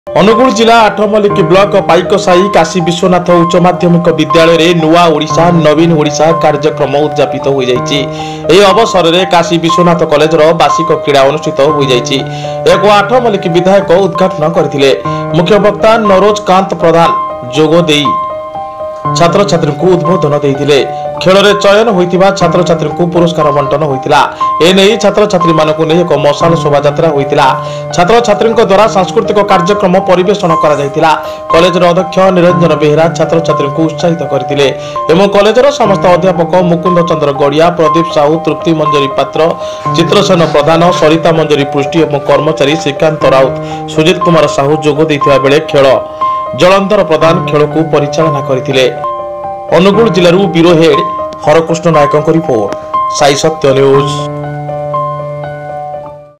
ଉଚ୍ଚ ମାଧ୍ୟମିକ ବିଦ୍ୟାଳୟ ରେ ନୂଆଁ ଓଡିଶା ନବୀନ ଓଡିଶା କାର୍ଯ୍ୟ କ୍ରମ ଉଦଯାପିତ ଦିବସ ||SAI SATYA NEWS
ଉଚ୍ଚ ମାଧ୍ୟମିକ ବିଦ୍ୟାଳୟ ରେ ନୂଆଁ ଓଡିଶା ନବୀନ ଓଡିଶା କାର୍ଯ୍ୟ କ୍ରମ ଉଦଯାପିତ ଦିବସ ||New Odisha New Odisha Work Order Implementation Day in Higher Secondary School. ||